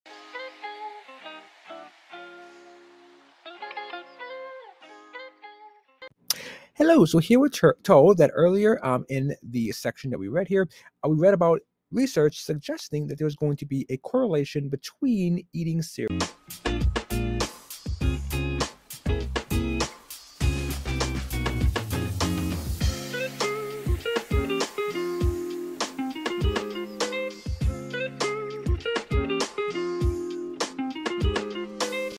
Earlier in this section, we read about research suggesting that there is a correlation between eati…
Earlier in this section, we read about research suggesting that there is a correlation between eating cereal and weight. Cereal companies that present this information in their advertisements could lead someone to believe that eating more cereal causes healthy weight. Why would they make such a claim and what arguments could you make to counter this cause-and-effect claim?